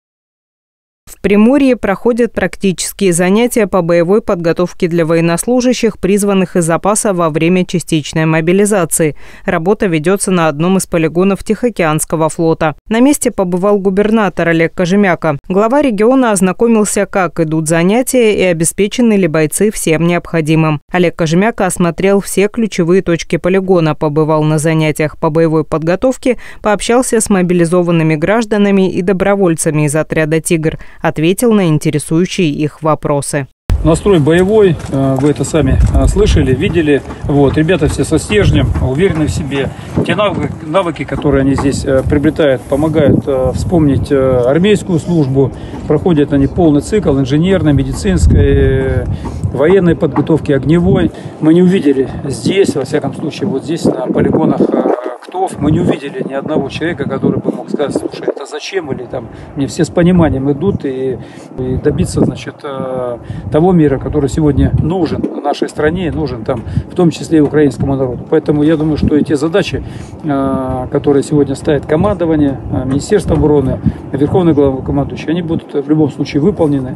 Олег Кожемяко проверил условия подготовки мобилизованных приморцев.
В Приморье проходят практические занятия по боевой подготовке для военнослужащих, призванных из запаса во время частичной мобилизации. Работа ведется на одном из полигонов Тихоокеанского флота. На месте побывал губернатор Олег Кожемяко.